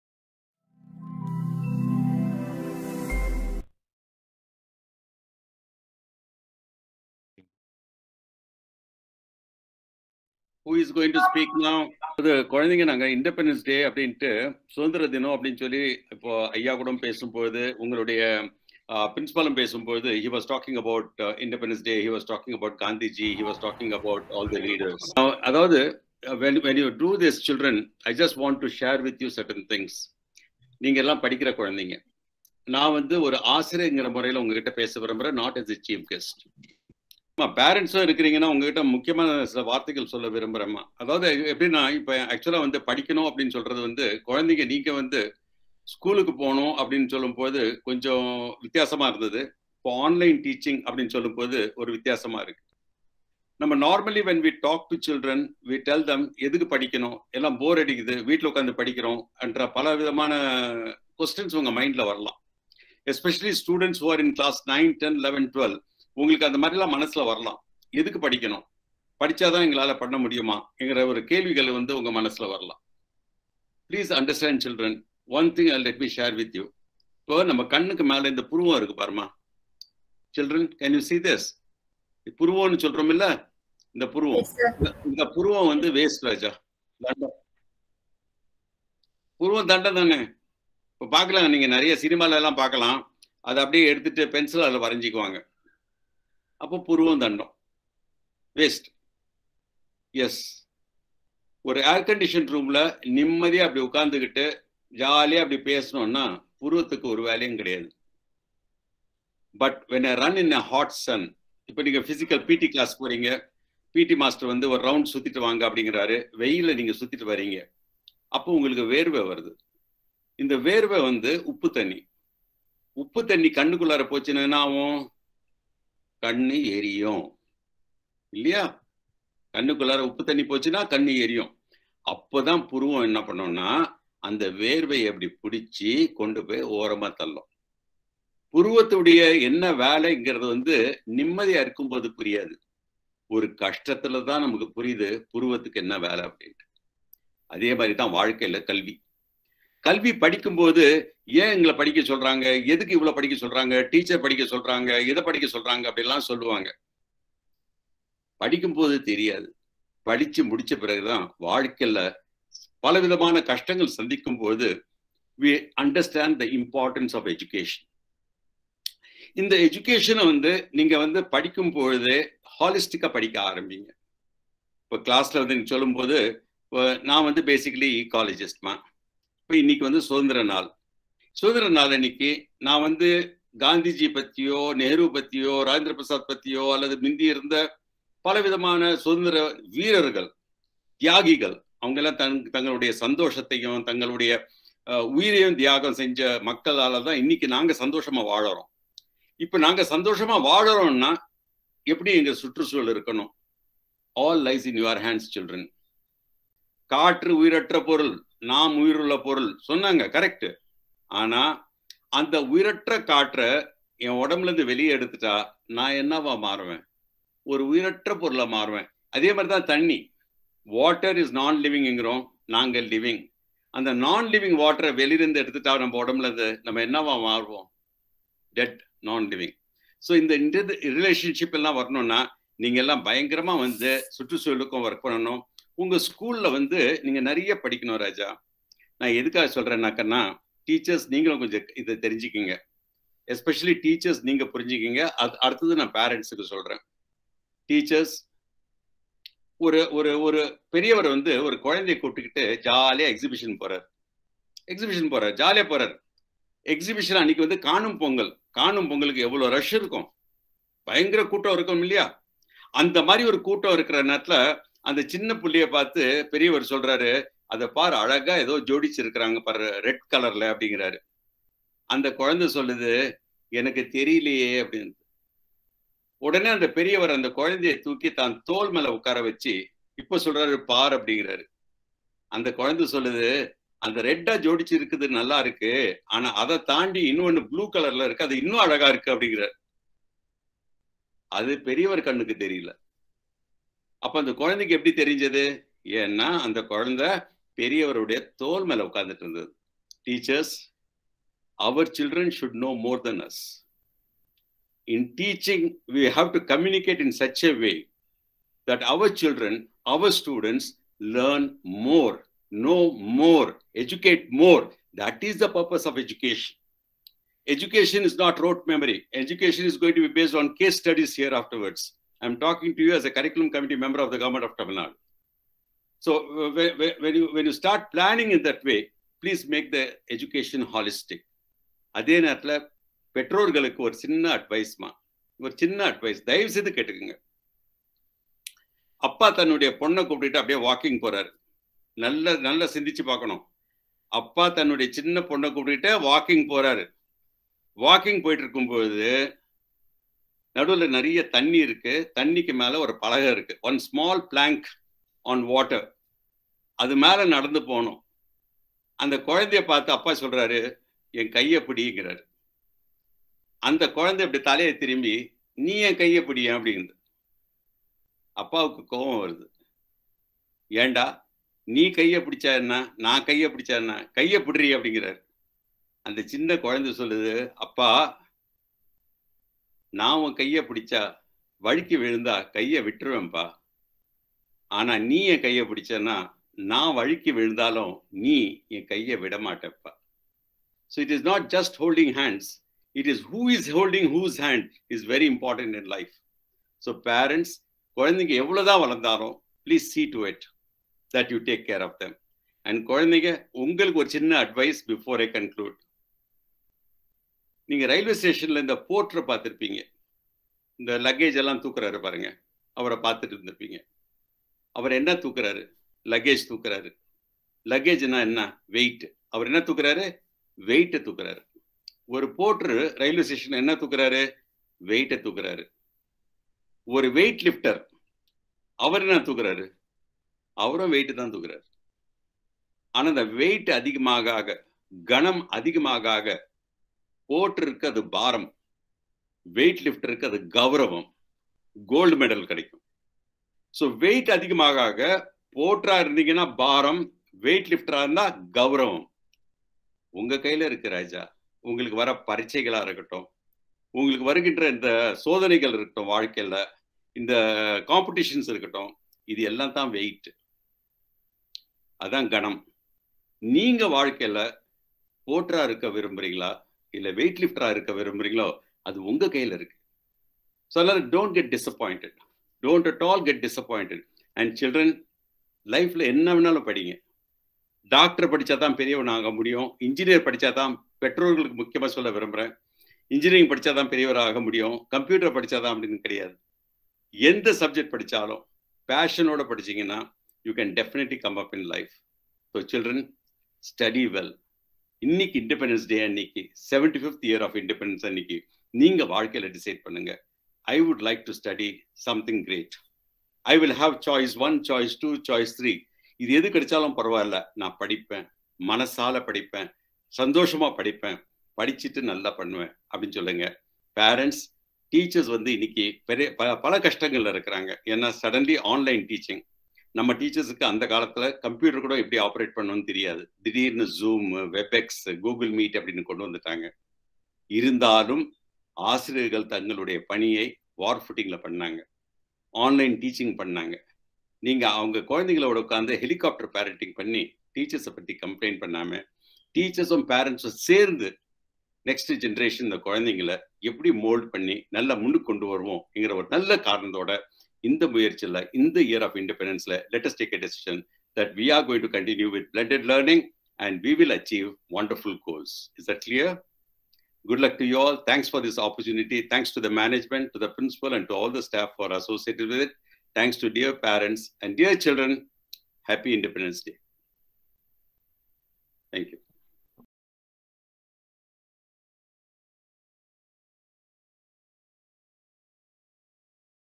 Independence Day (2021) - Chief Guest Speech
It’s really a pride moment for PPS to celebrate our 75th Independence Day with the whole nation even during pandemic situation in a complete way along with the support of teachers, students and parents with some excellent speech given by our students, an inspiring delivery from Senior Principal and a motivational speech by Our Guest of Honor Dr. Sultan Ahmed Ismail which attracted the entire audience through online.